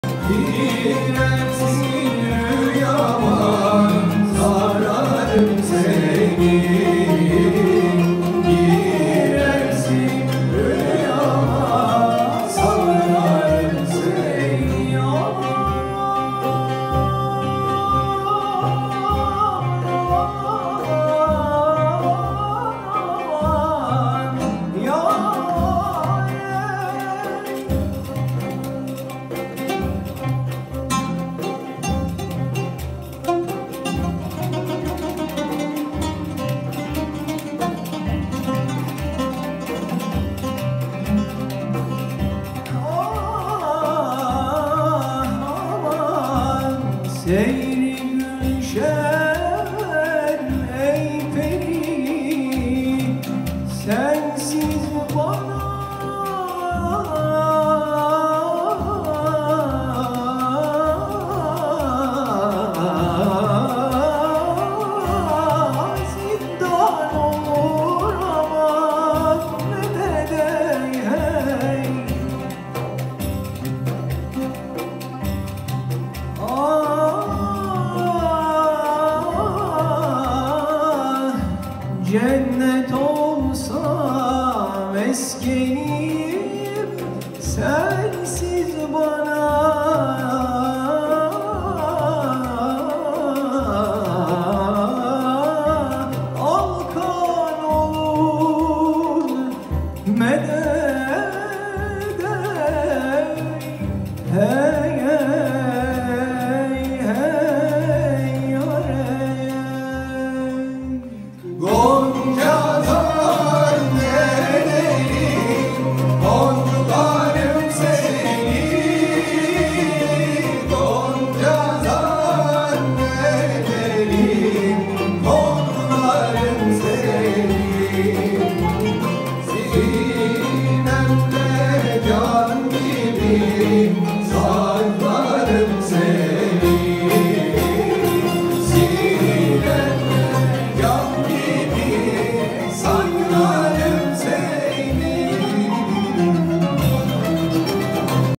Ahmet Yağmur Kucur - Gazel
2 Şubat 2024 tarihinde AKM’nin Çok Amaçlı Salon’unda İstanbul Tarihi Türk Müziği Topluluğu sanatçıları olarak verdiğimiz konserden, kürdilihicazkâr faslının arasına düşürdüğüm gazel.